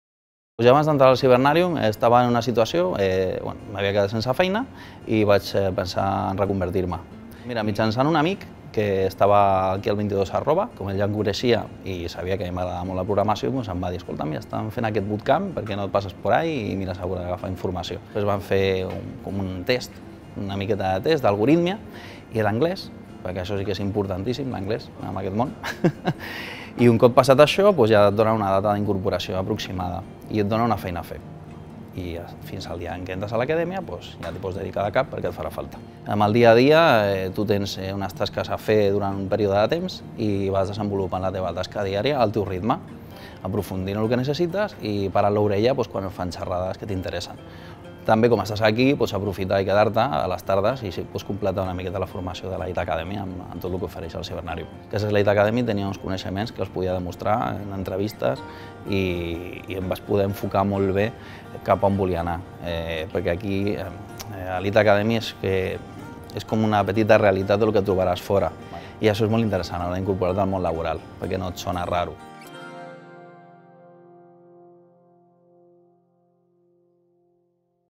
Xavier Roldán - Alumne IT Academy
En Xavier Roldán va ser alumne d'IT Academy de Cibernàrium i, actualment, és programador júnior. → Si tens curiositat pel món de la programació, vine a formar-te a IT Academy